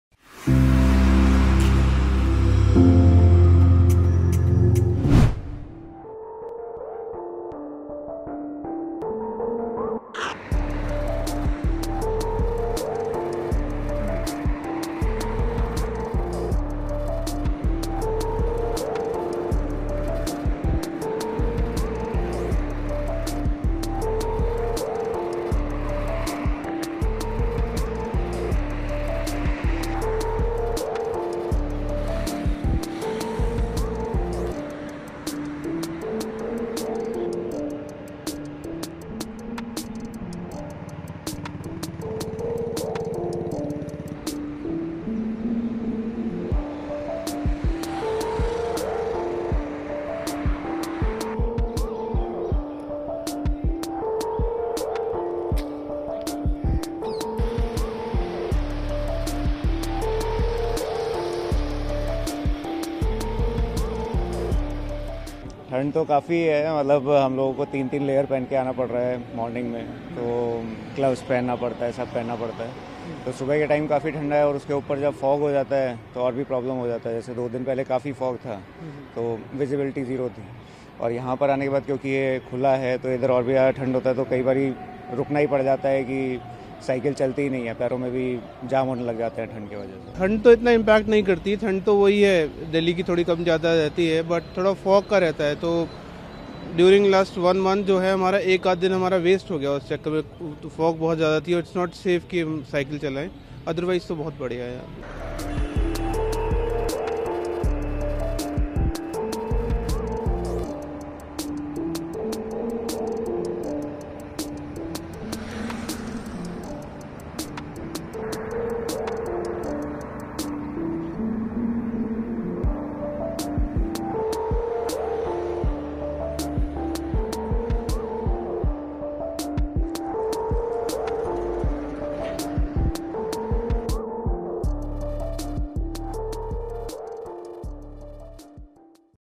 Delhi continues to shiver under cold wave
Dense fog shrouded the national capital reducing the visibility in the morning of 18 January. Cold waves blowing across city left Delhiites shivering. Severe winters continued to tighten its grip over northern India. One of the cyclists said, 'We are wearing three layers and gloves because of severe cold and fog in the morning.' They also complained that some days where fog increases, it becomes difficult to ride bicycles. According to the Indian Meteorological Department, the national capital recorded temperature of 11.2 degree Celsius in the morning.